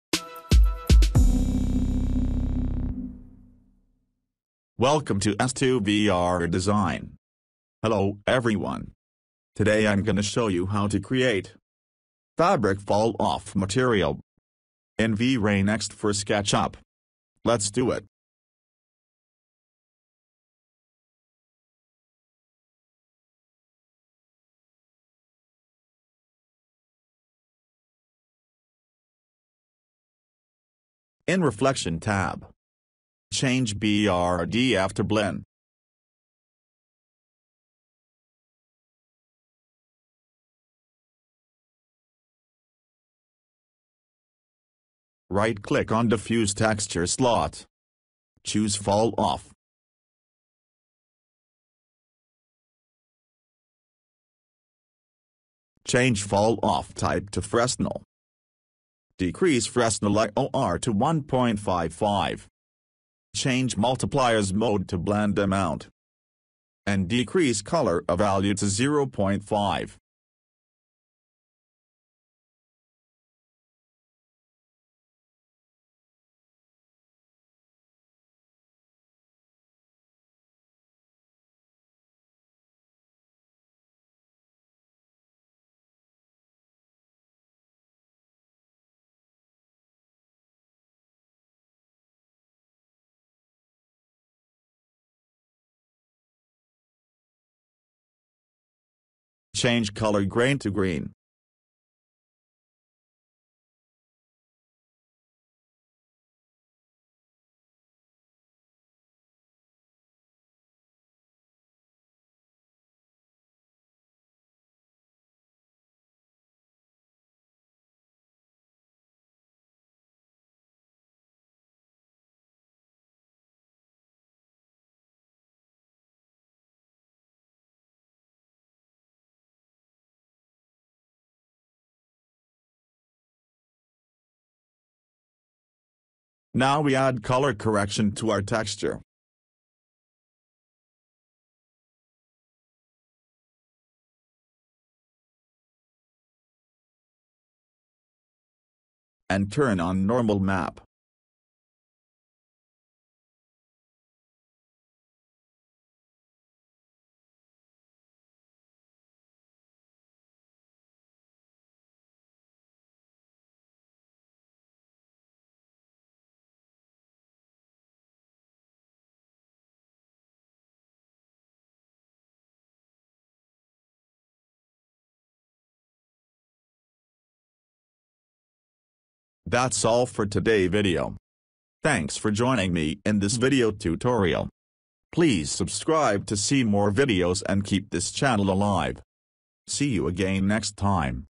Fabric Falloff Material in V-Ray Next for SketchUp
In this video, I am going to show you how to create Fabric Falloff Material in V-Ray Next for SketchUp.